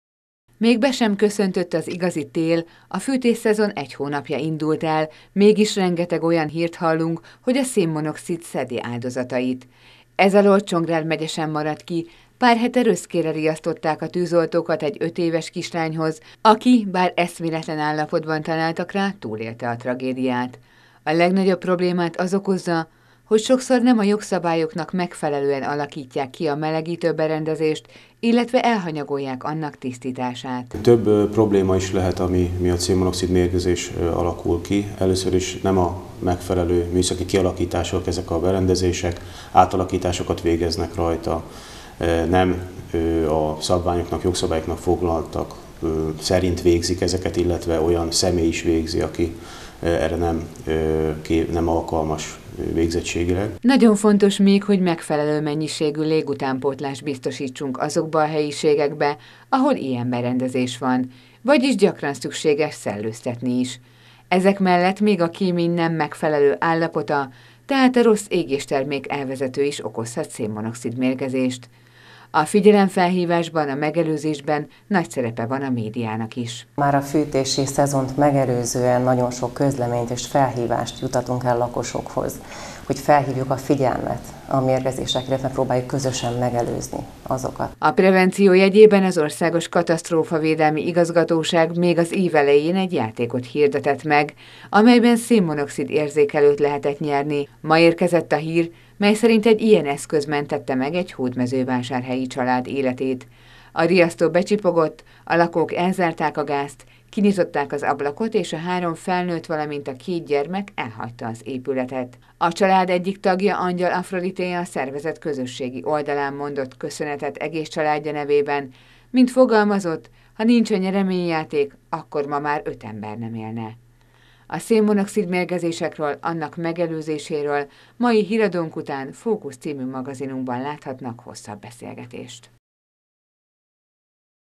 Csendes gyilkos
Nemrég kezdődött meg a fűtési szezon, a csendes gyilkos, vagyis a szén-monoxid máris szedi áldozatait. Megyénkben is történt ilyen eset októberben, amikor csak a szerencsén múlt, hogy nem szenvedett halálos mérgezést egy ötéves röszkei  kislány. A gyakran tragikus kimenetelű események többsége ugyanakkor kellő odafigyeléssel és felkészüléssel megelőzhető lenne.